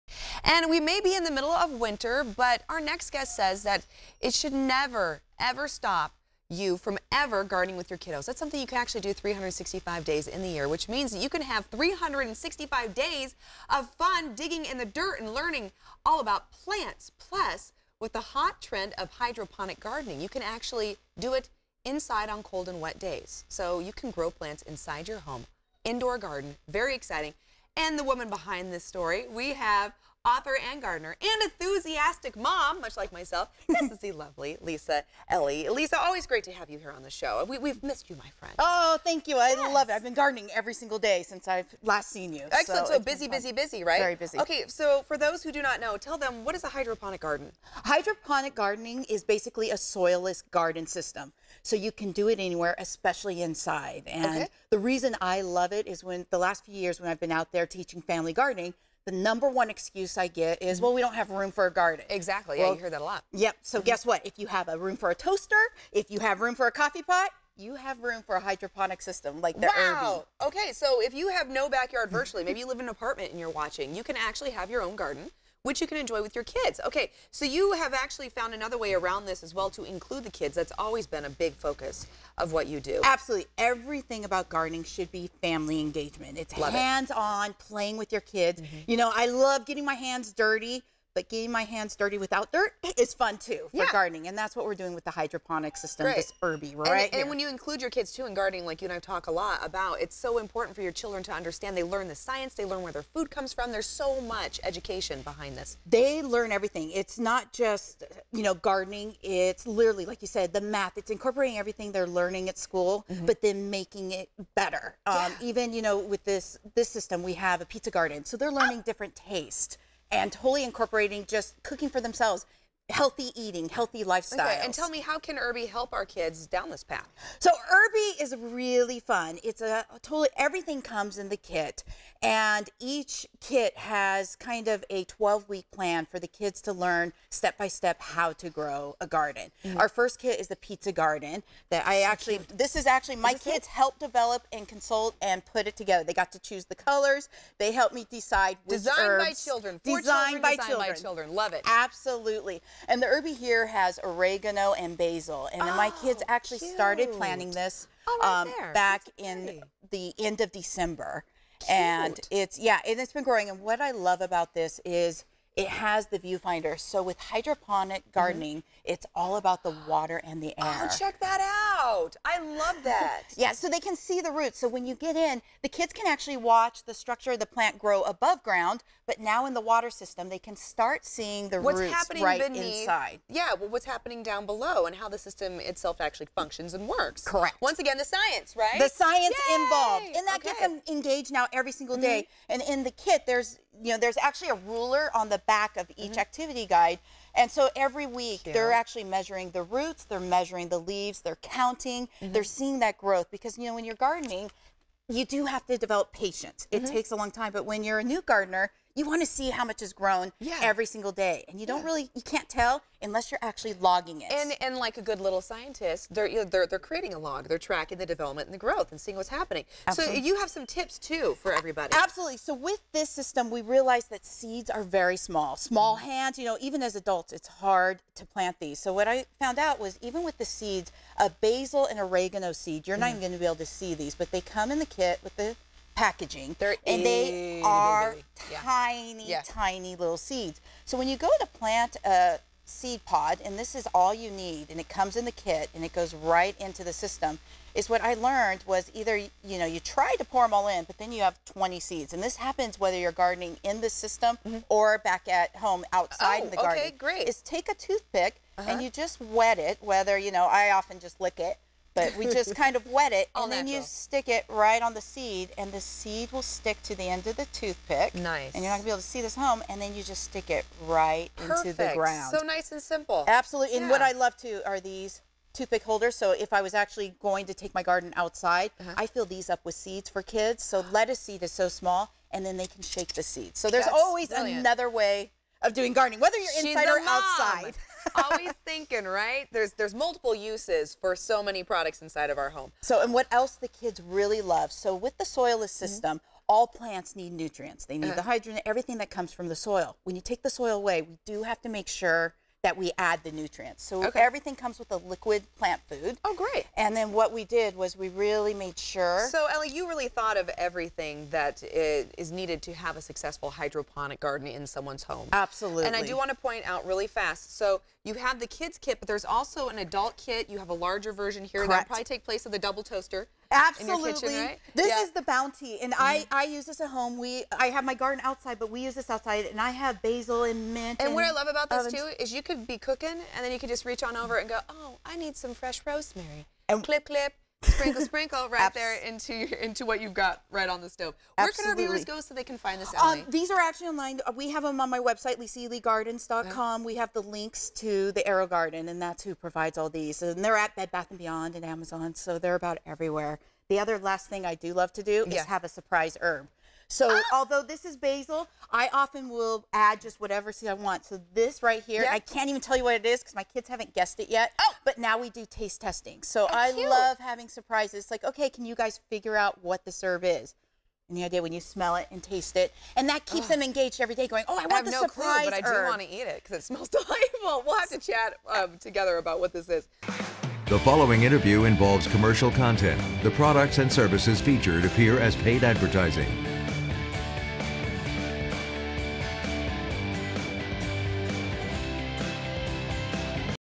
Hydroponic for Kids
Garden with your kids indoors with Lisa Ely's hydroponics kit!